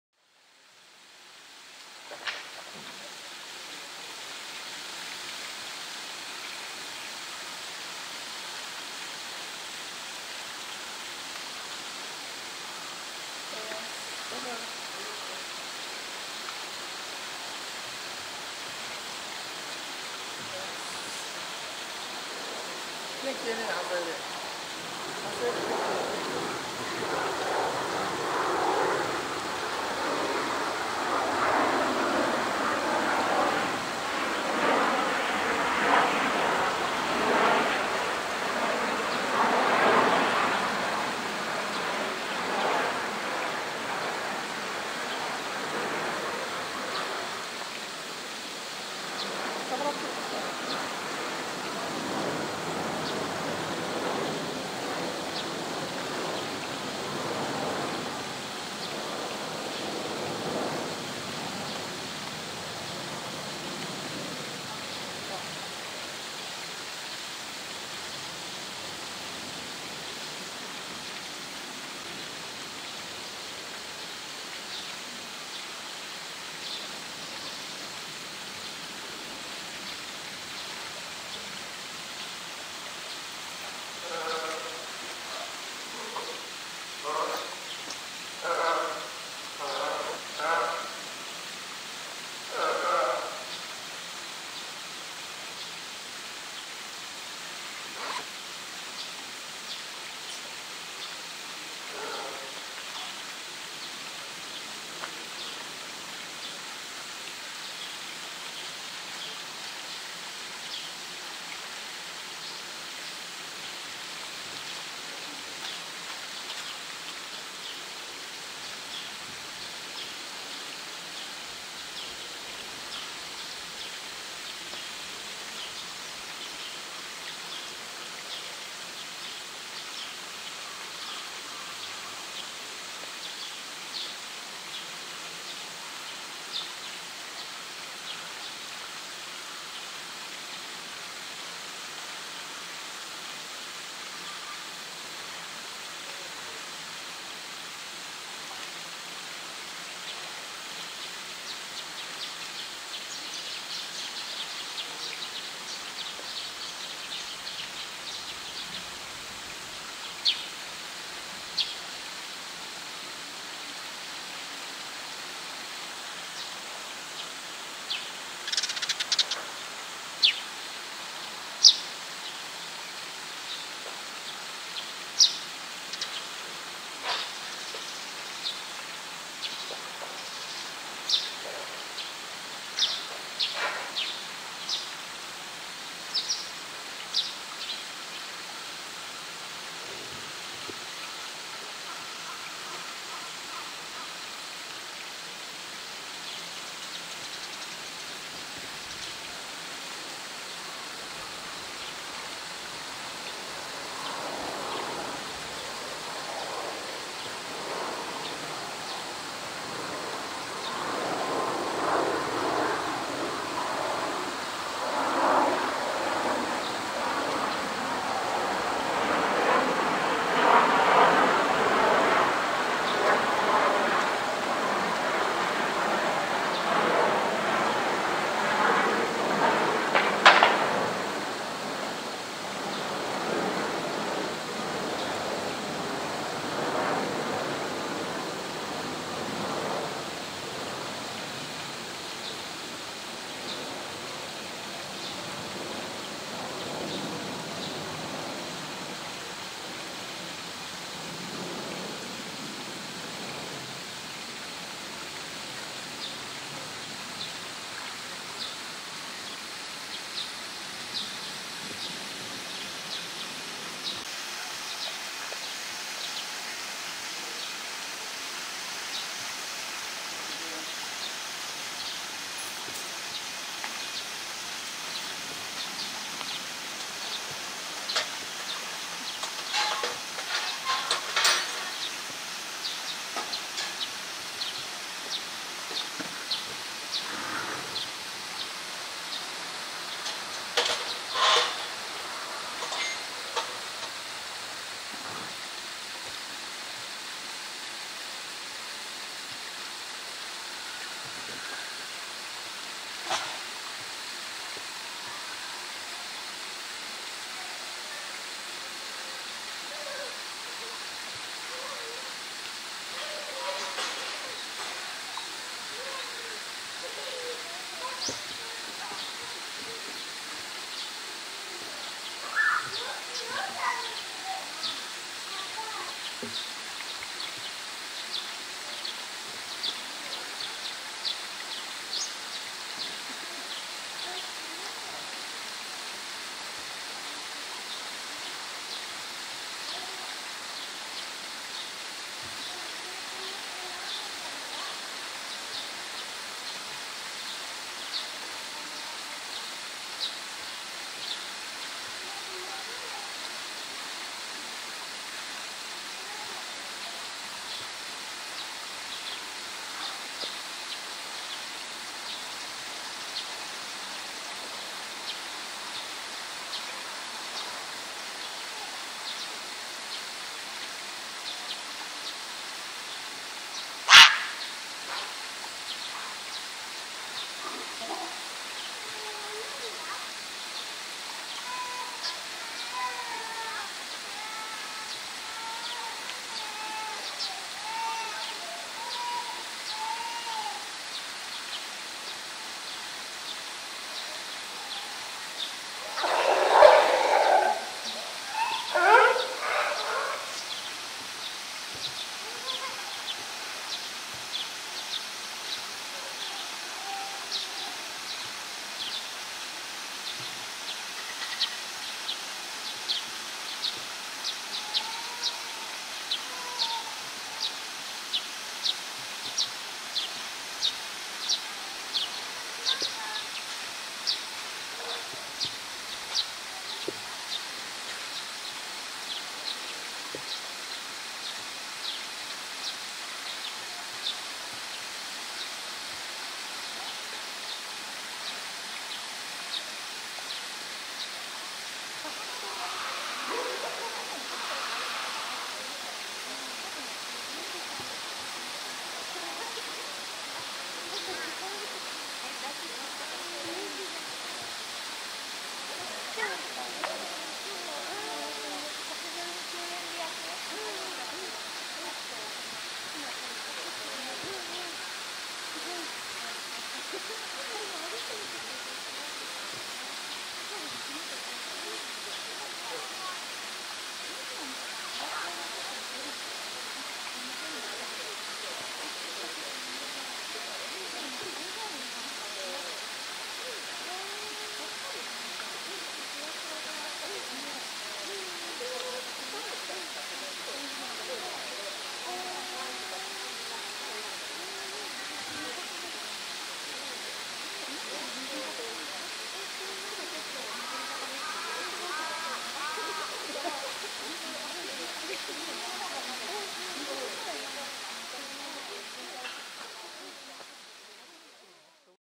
朝一番のフブキ！【東山動物園】2023/05/23 火曜日 AM 9:20頃､撮影 🎥
2023年5月23日 火曜日 開園と共に白くま舎へ向かい撮影したフブキの様子です。

プールに飛び込んだ後の様子です。

Tuesday, May 23, 2023 This is a picture of Fubuki taken at the opening of the park and heading to the white bear house.

This is what it looks like after jumping into the pool.